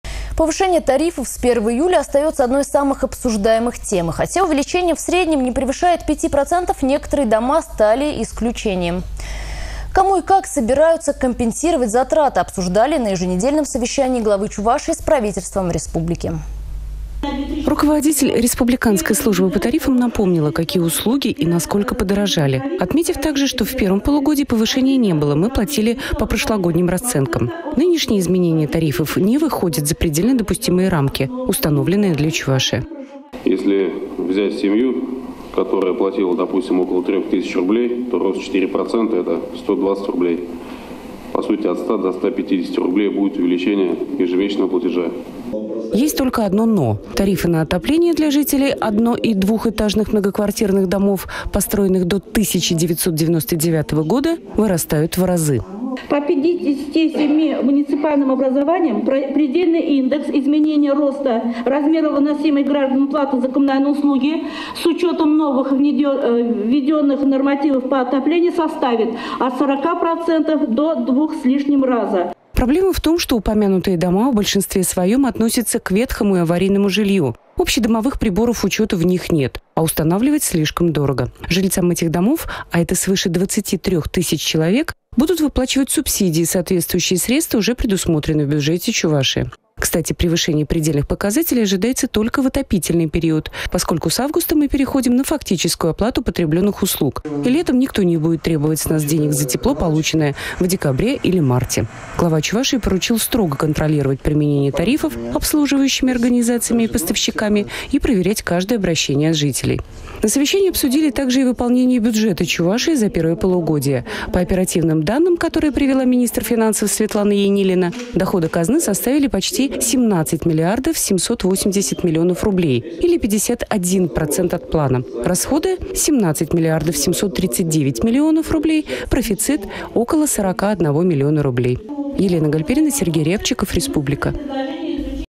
Тарифы под контролем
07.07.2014
Глава Чувашии Михаил Игнатьев провел совещание с Правительством республики.